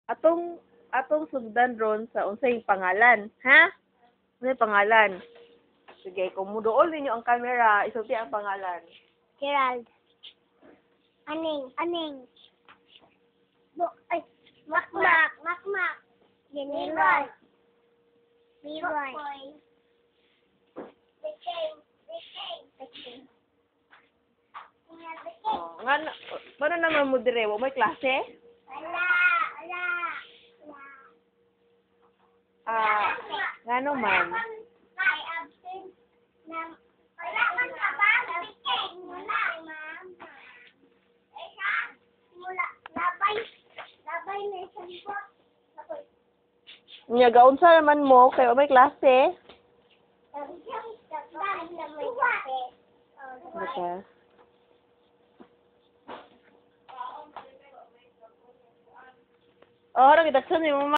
kids in the silingan no classes
video them even i cant see on the screen, cp lights went crazy, sometimes it will be on sometimes not.